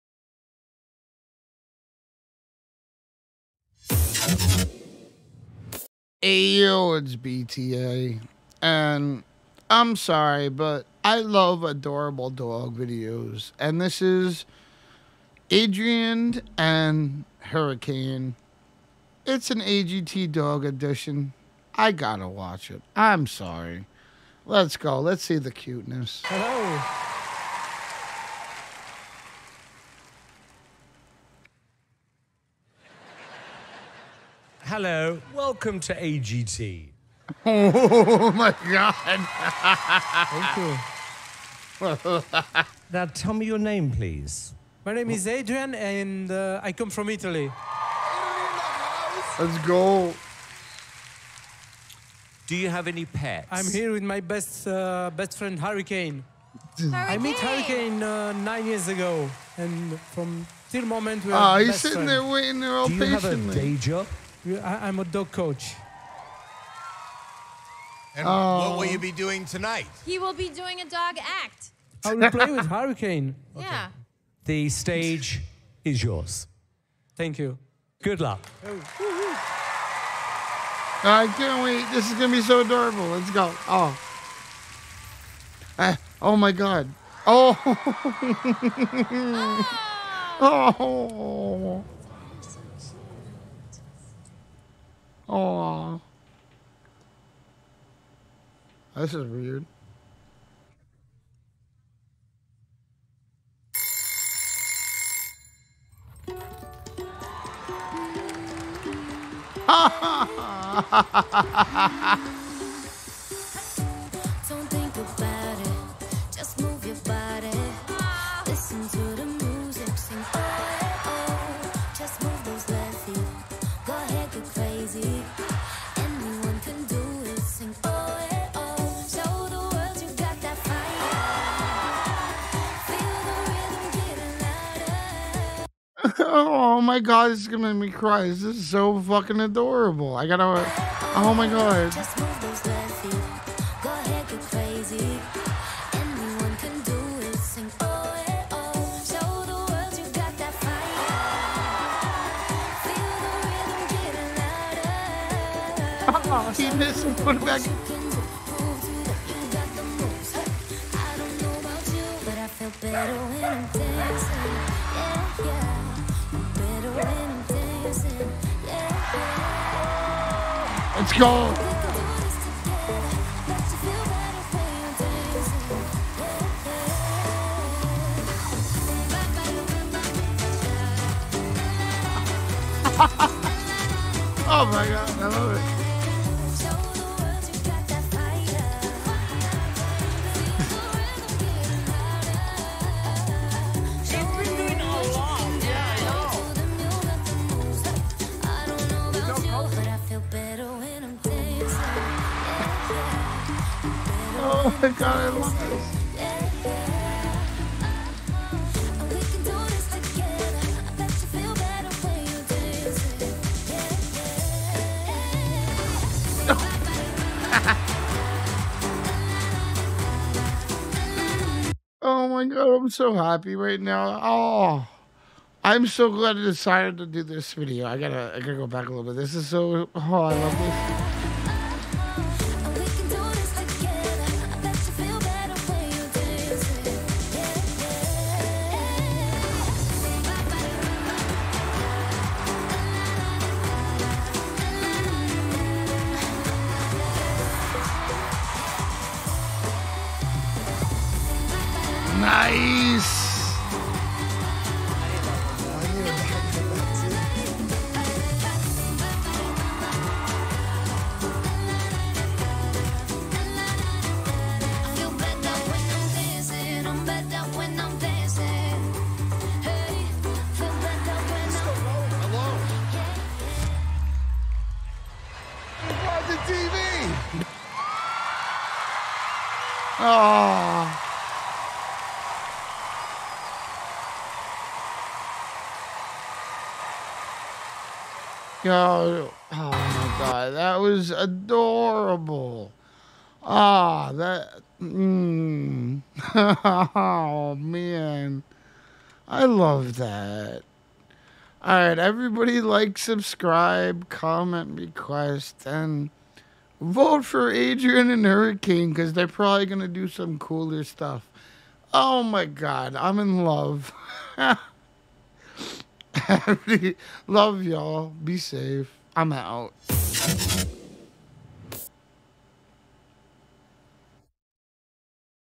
Adrian Stoica & Hurricane deliver heartwarming moments and ADORABLE tricks .. Bta reaction
Bta review
Adrian Stoica & Hurricane deliver heartwarming moments and ADORABLE tricks
love yall